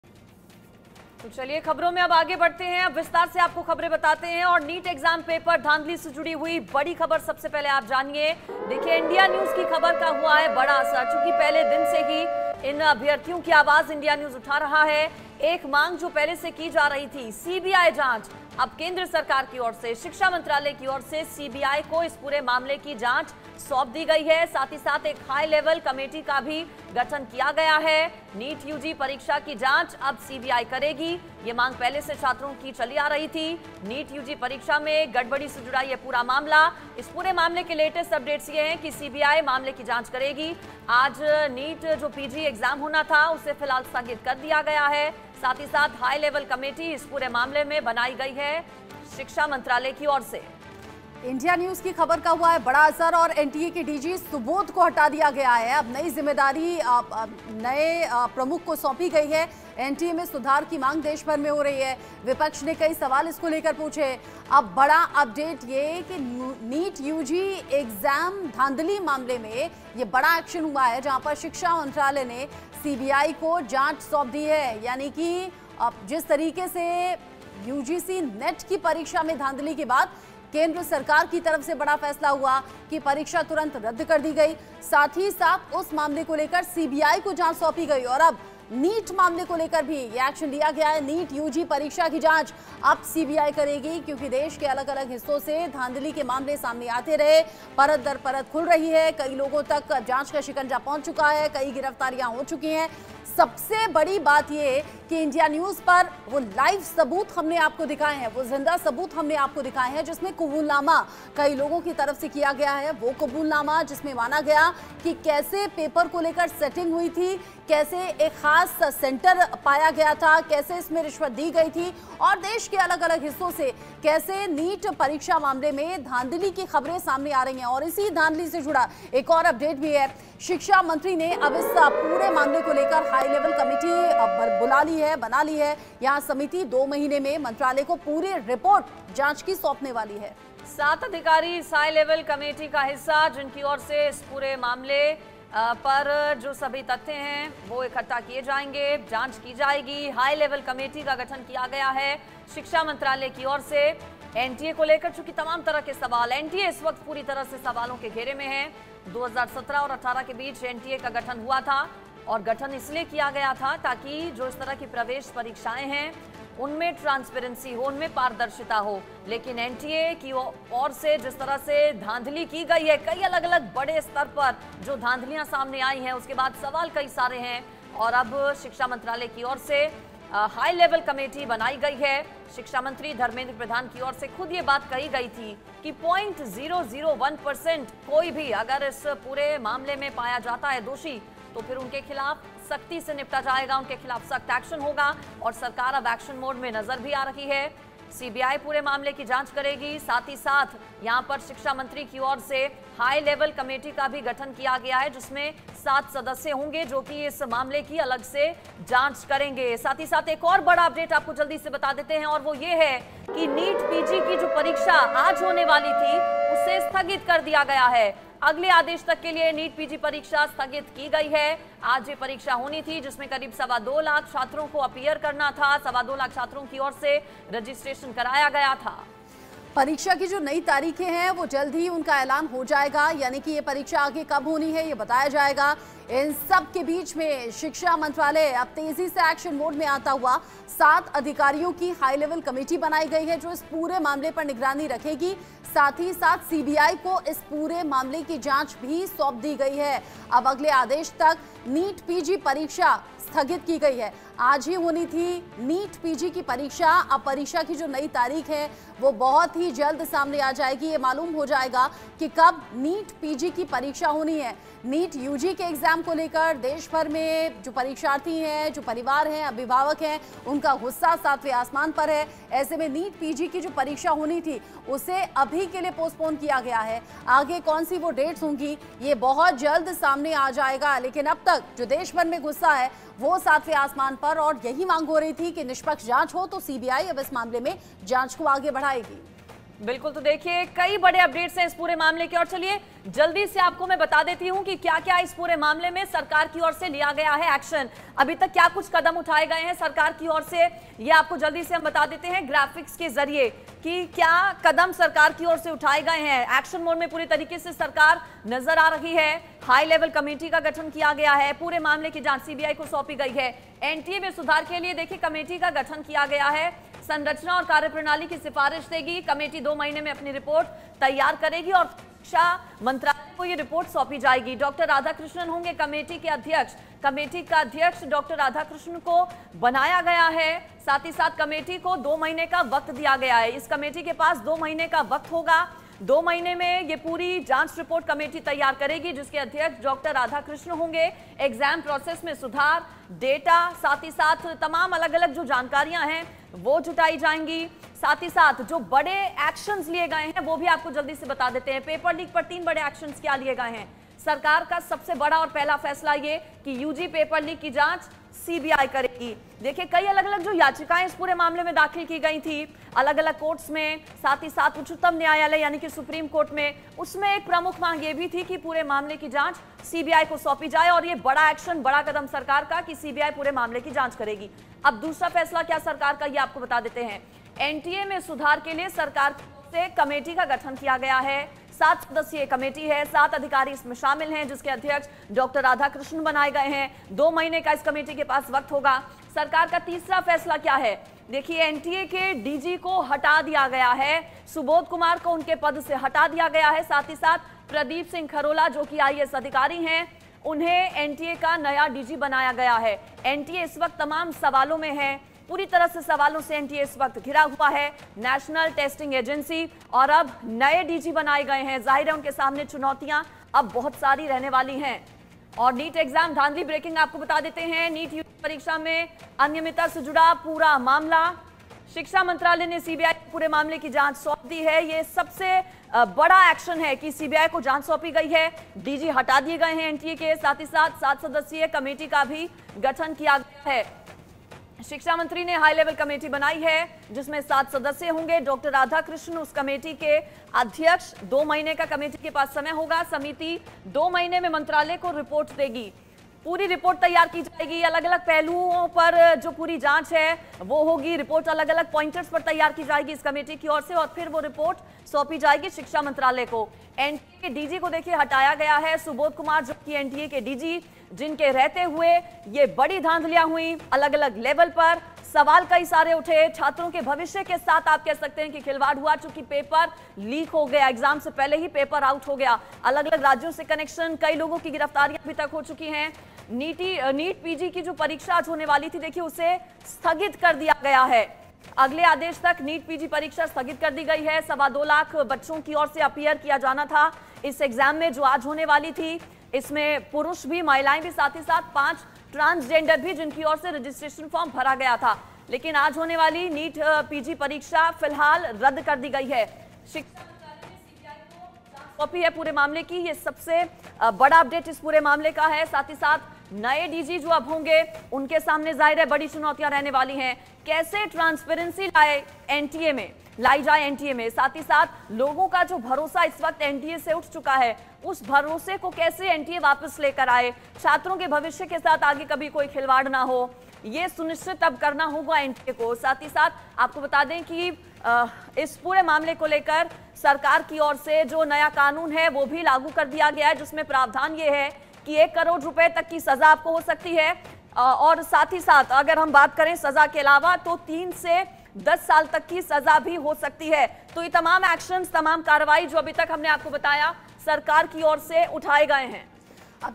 NEET-PG Paper Cancelled Updates : एग्जाम से एक दिन पहले NEET-PG की परीक्षा स्थगित | NTA
नीट-यूजी पेपर लीक मामले में शिक्षा मंत्रालय ने बड़ा फैसला किया है। NEET-UG पेपर लीक मामले की जांच CBI को सौंपी गई है। इससे पहले खबर आई कि रविवार (23 जून) को आयोजित की जाने वाली नीट-पीजी की प्रवेश परीक्षा स्थगित कर दी गई। अब इस परीक्षा के लिए नई तारीख का ऐलान किया जाएगा।

The Ministry of Education has taken a big decision in the NEET-UG paper leak case. The investigation of the NEET-UG paper leak case has been handed over to the CBI. Earlier, news came that the NEET-PG entrance exam to be held on Sunday (23 June) was postponed. Now a new date will be announced for this exam.